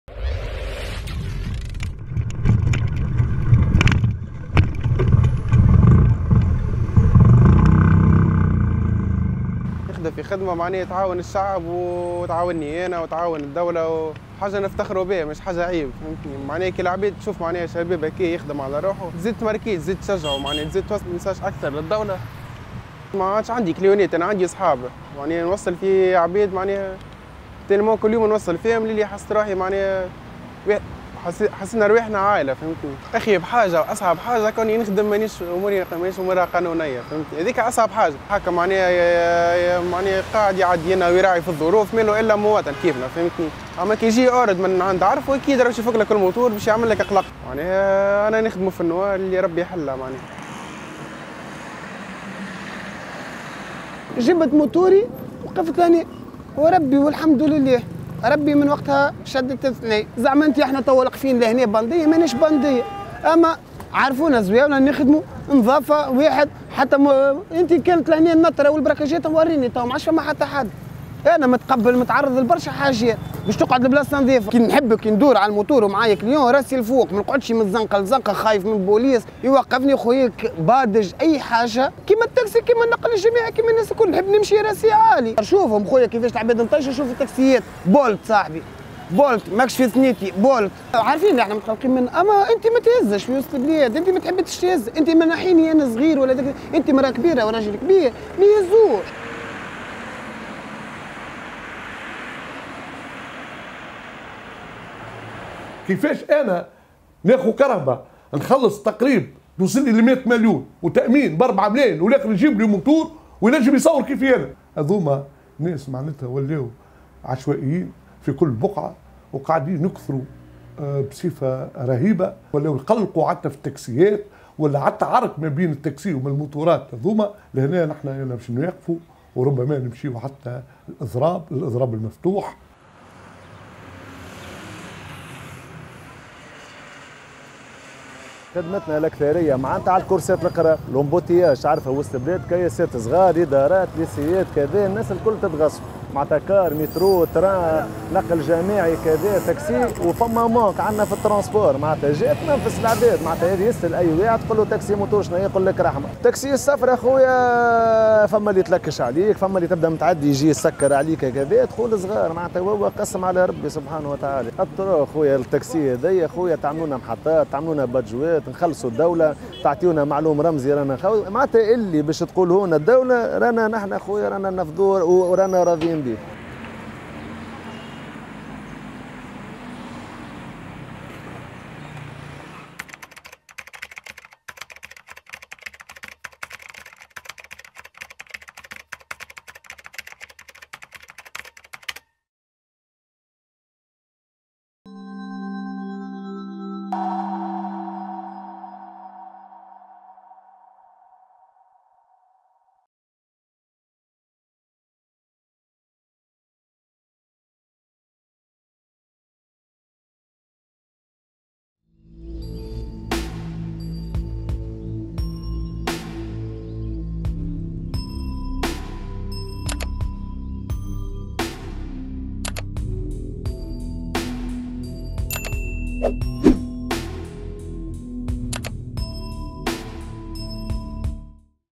دراجات "التاكسي موطو": في انتظار حل قد لا يأتي
أمام تردي خدمات النقل العمومي بالعاصمة والاختناق المروري الذي تعرفه طرقها خلال أوقات الذروة، راجت خدمة النقل عبر الدراجات النارية كحل وقتي غير منظم. ورغم فشل تجارب تقنينها والتوتر الذي خلفته مع منافسيها، تواصل السلطات لف الموضوع بما تيسر من صمت وغموض.